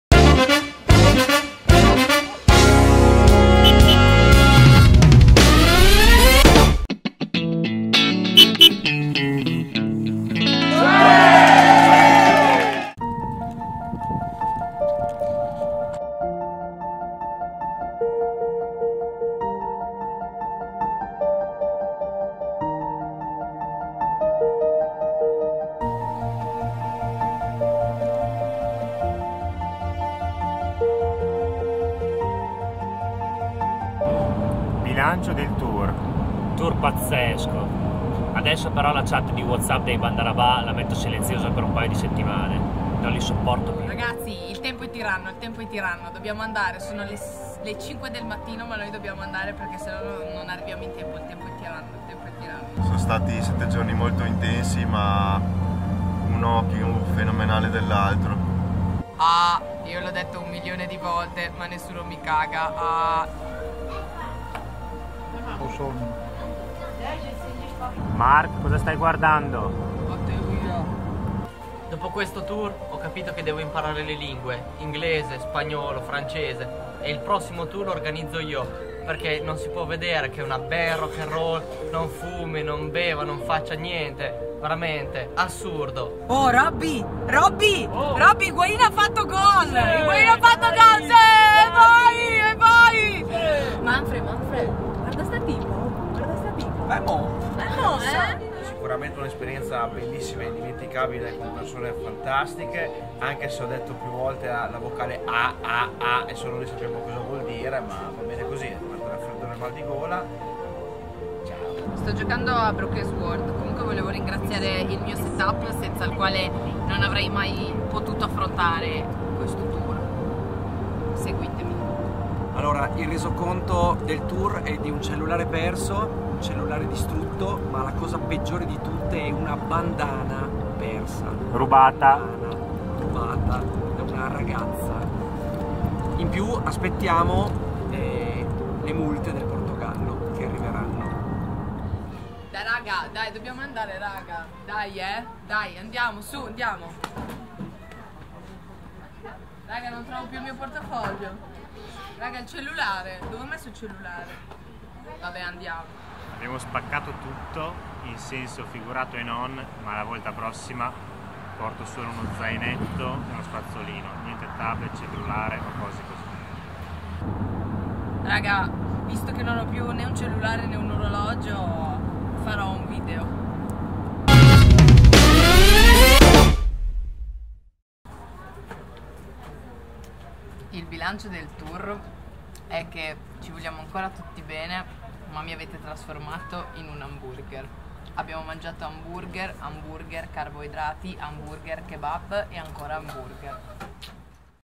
TOURIBERICO 2018 DAY7
Video diary del tour che ci a portati da Reggio Emilia fino a Coimbra, Portogallo!
Risate, tanta musica e divertimento!
Giorno Uno, in vaiggio da Barcellona a Reggio!
Il bilancio del tour!